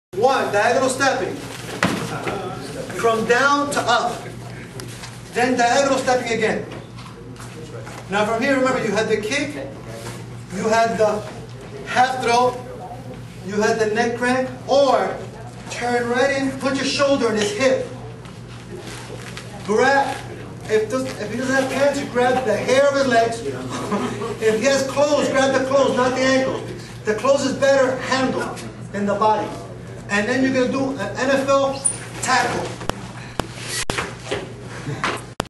NMS Tampa Workshop DVD preview
NMS DVD preview of a seminar in tampa florida showing strategic stepping crossing and throwing